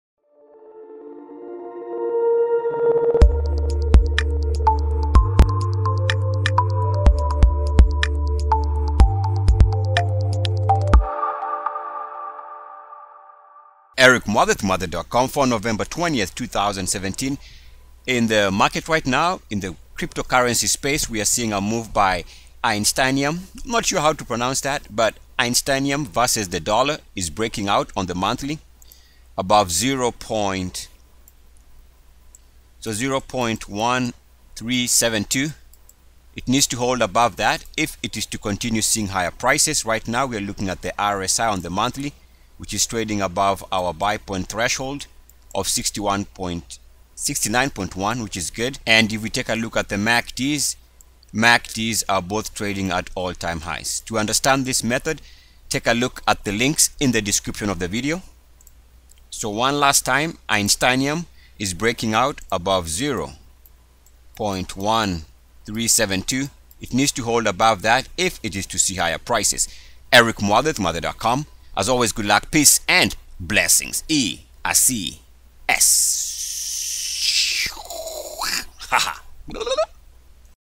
How To Trade CryptoCurrency - Einsteinium / Dollar - EMC2USD
Einsteinium / Dollar - EMC2USD - is currently clearing the breakout level of $0.1372 on its monthly chart.

how to trade cryptocurrency reddit
Cryptocurrency trading for beginners
Cryptocurrency trading sites
Cryptocurrency trading strategy